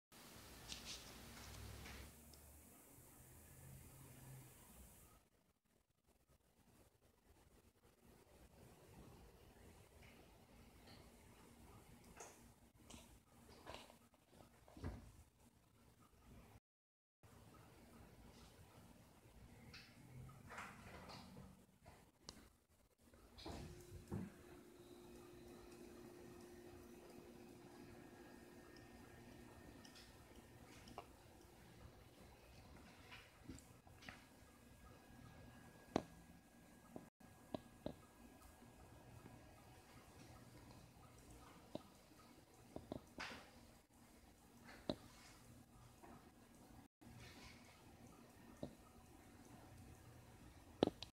Haunted Microscope with littleBits!
Make a haunted microscope using littleBits!

This project teaches the basics of using a microscope as well as preparing slides.